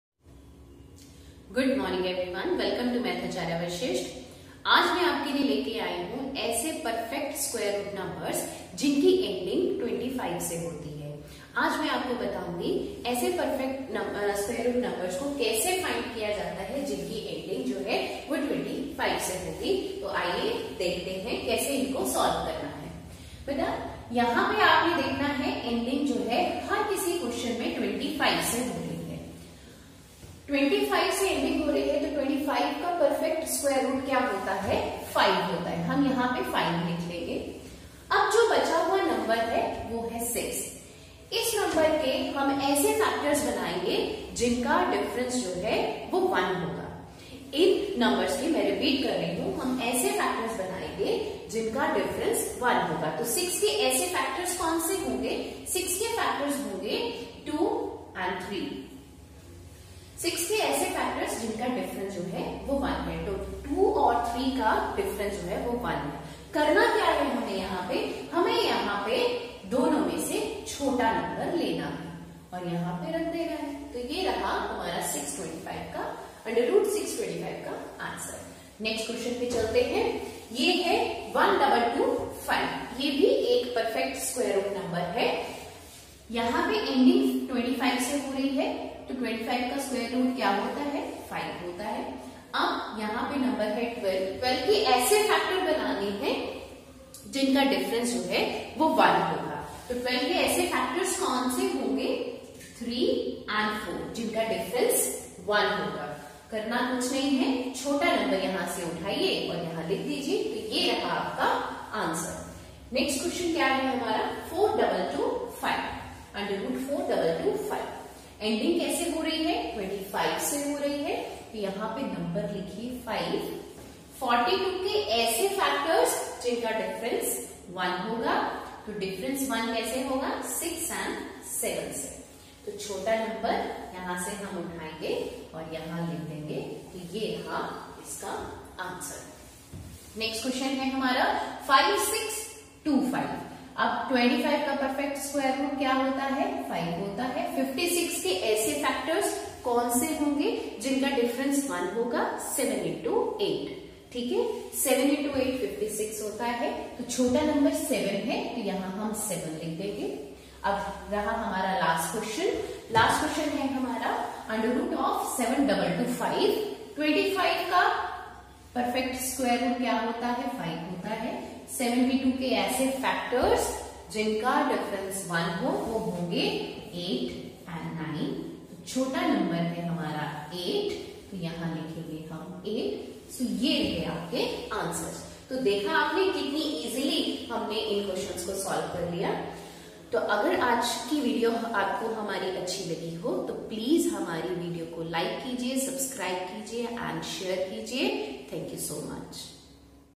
Square root tricks in just 3 seconds. square root of special no's ending with 25.
This video is related to all competitive exams like Bank Po, Railways, SSC, police Cds etc.